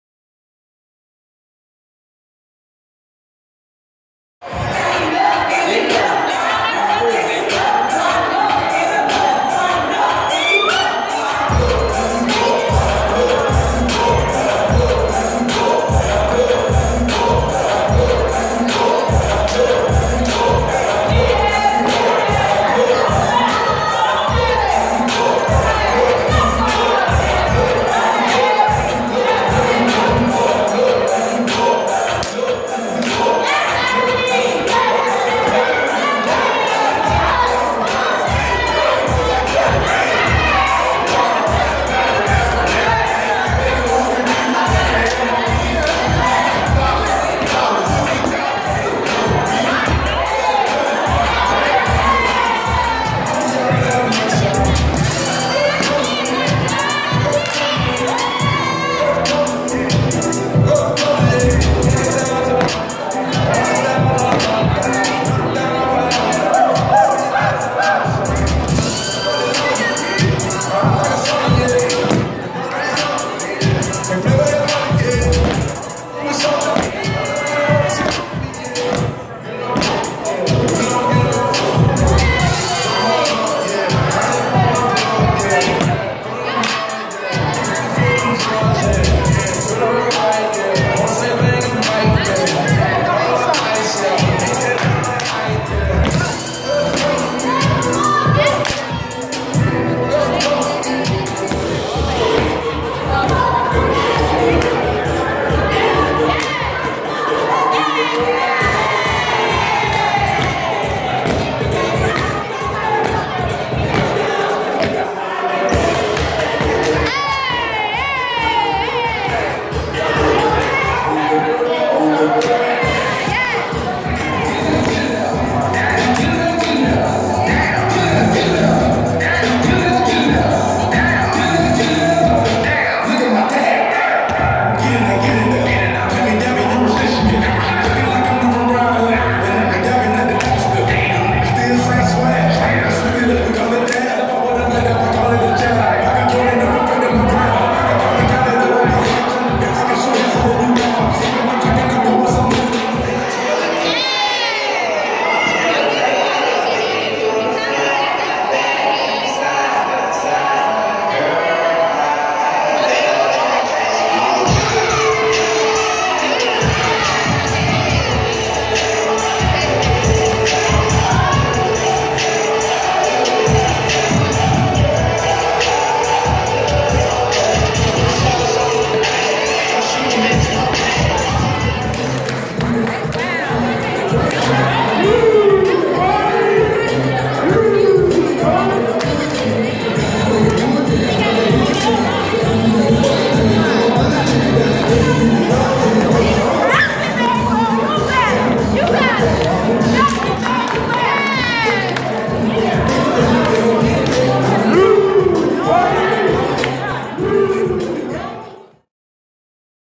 UNIVERSITY OF THE DISTRICT OF COLUMBIA GREEKS STROLLING
The University of the District of Columbia held a Greek stroll for the students during the Extravaganza in the student center on January 21, 2016.